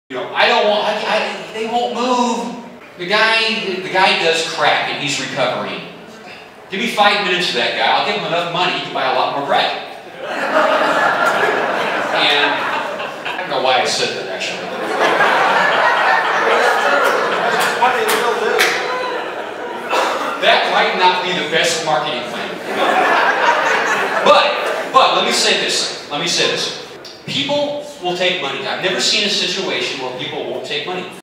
David Oldenburg Speaking 09/04/2018 Real Estate Investing
This is just me having fun with my big real estate investing group.  Interested in learning more or coming to a live event? RosevilleMeetup.com we have FREE meetings every month with a packed house!